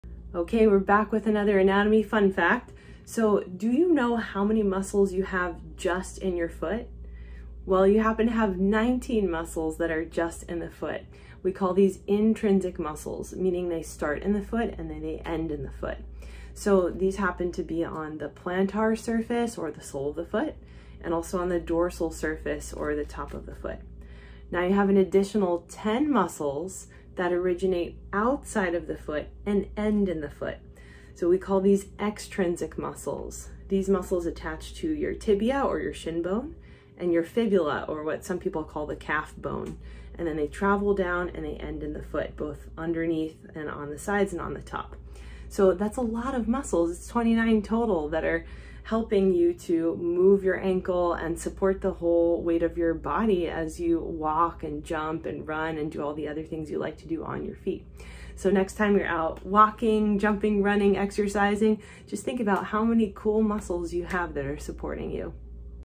Anatomy fun fact: muscles of the lower leg and foot
That’s a lot of muscles 🤯

Share this fun fact with a friend and get up and get those muscles moving!